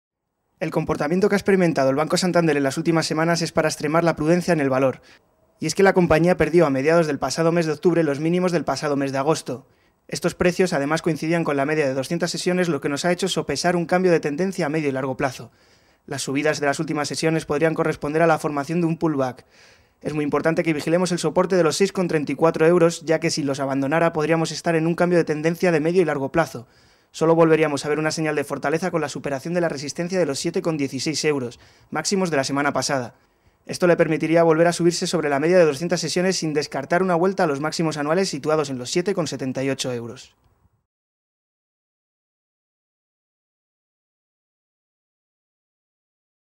Vídeo análisis técnico del Banco Santander
El comportamiento que ha experimentado el Santander en las últimas semanas es para extremar la prudencia en el valor.

Y es que la compañía perdió, a mediados del pasado mes de octubre los mínimos del pasado mes de agosto.